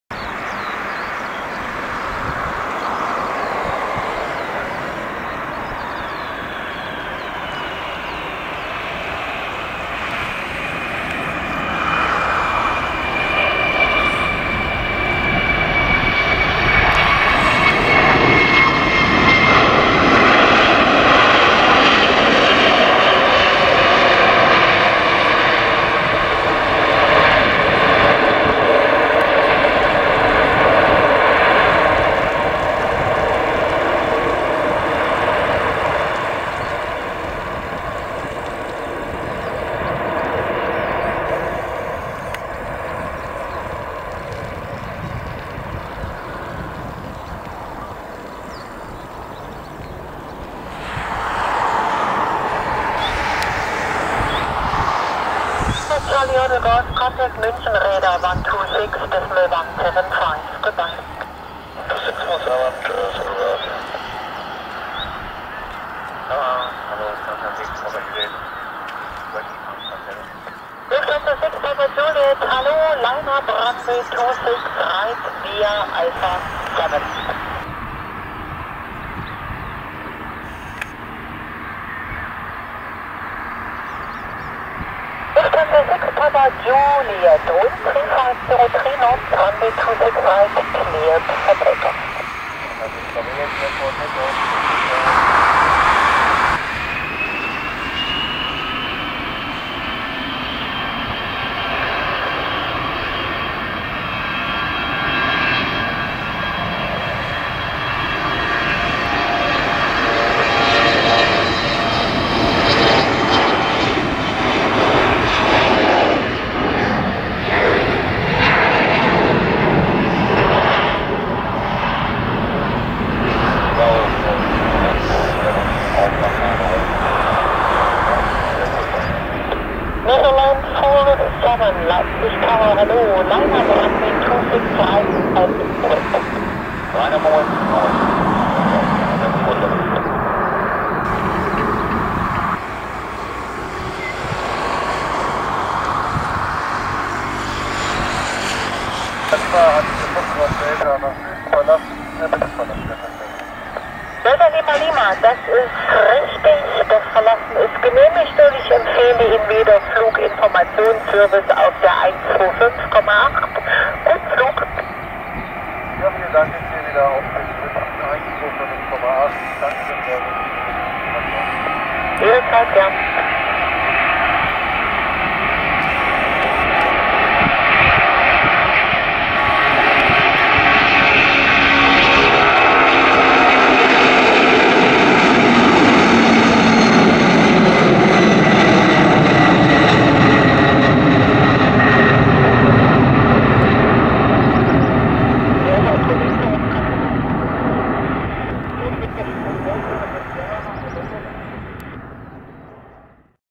Royal Netherlands Air Force McDonnell Douglas DC-10 T-264 @ LEJ/EDDP Leipzig/Halle Airport
Aircraft Type *  McDonnell Douglas DC-10-30
Date of Manufacture *  Oct 1978
Age  36.7 Years
Airframe Status  Active